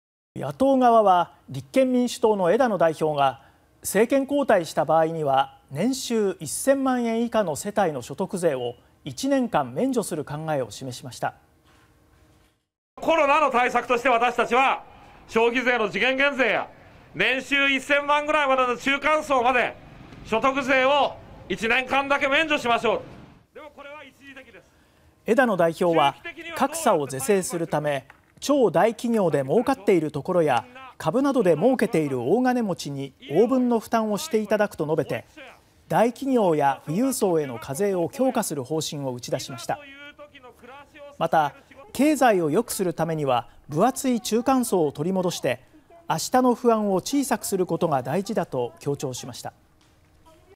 立憲・枝野代表「中間層まで所得税1年間免除」方針(2021年9月27日)
野党側は立憲民主党の枝野代表が、政権交代した場合には年収1000万円以下の世帯の所得税を1年間免除する考えを示しました。

　立憲民主党・枝野代表：「消費税の時限減税や年収1000万ぐらいの中間層まで、所得税を1年間だけ免除しましょう」

　枝野代表は格差を是正するため、「超大企業でもうかっているところや、株などでもうけている大金持ちに応分の負担をして頂く」と述べて、大企業や富裕層への課税を強化する方針を打ち出しました。

　また、「経済をよくするためには分厚い中間層を取り戻して、あしたの不安を小さくすることが大事だ」と強調しました。